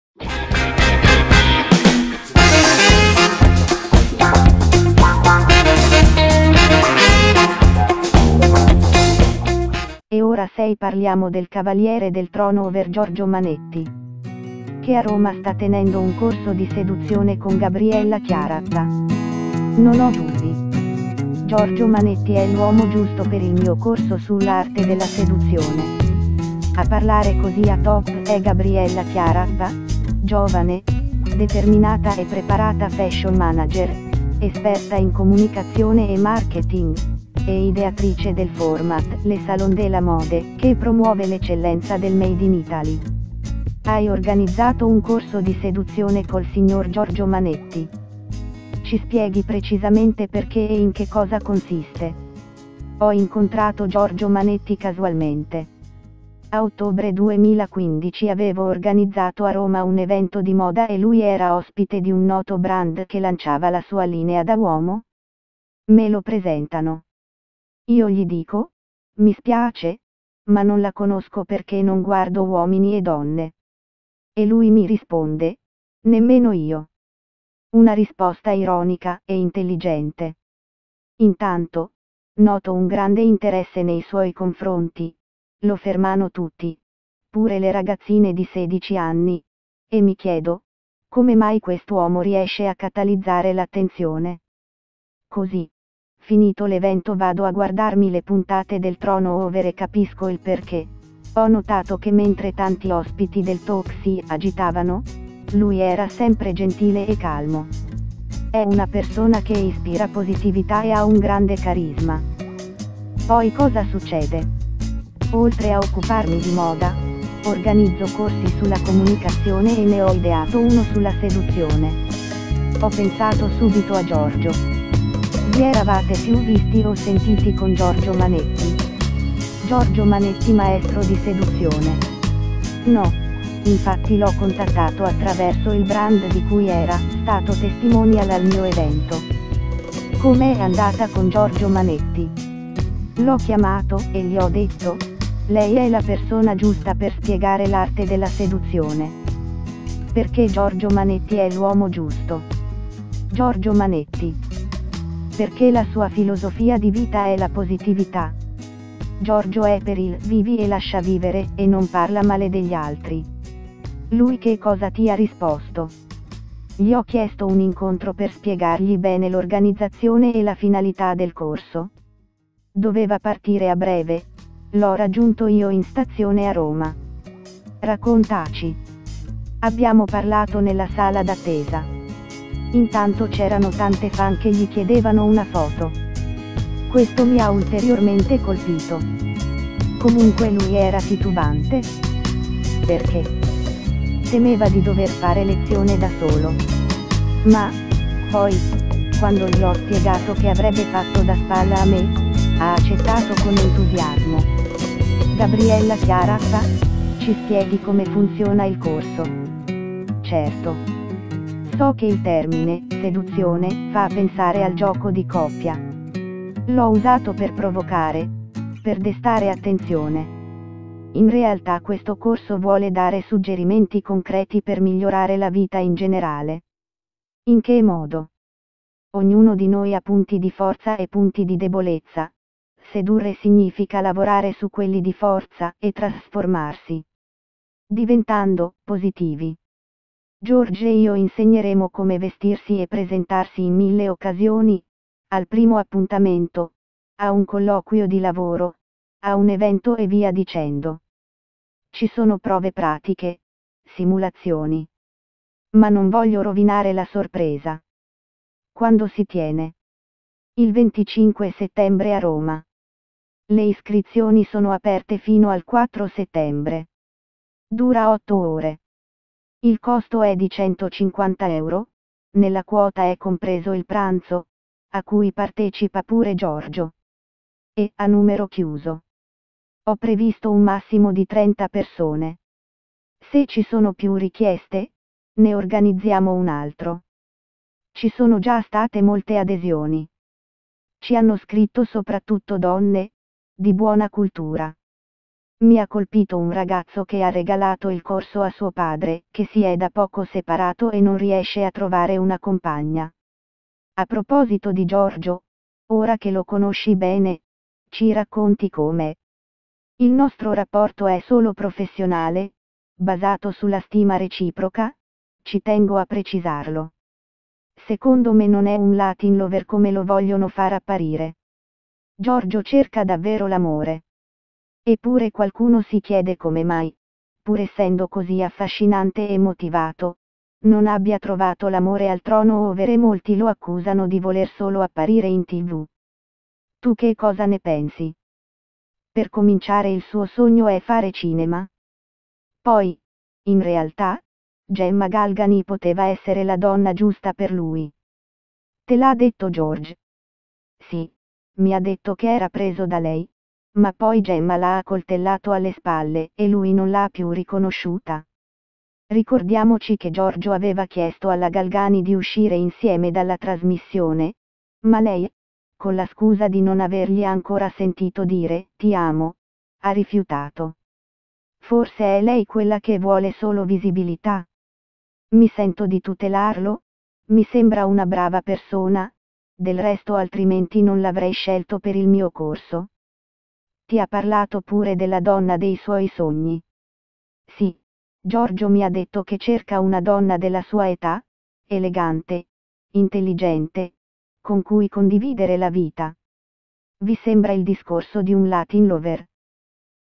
Giorgio Manetti maestro di Seduzione
E questo è un articolo dedicato a Giorgio Manetti che ora è diventato anche maestro di seduzione. Ovviamente confermatissimo per la nuova stagione del programma che sta per iniziare e non mancheranno quindi i confronti con Gemma Galgani. Articolo a cura di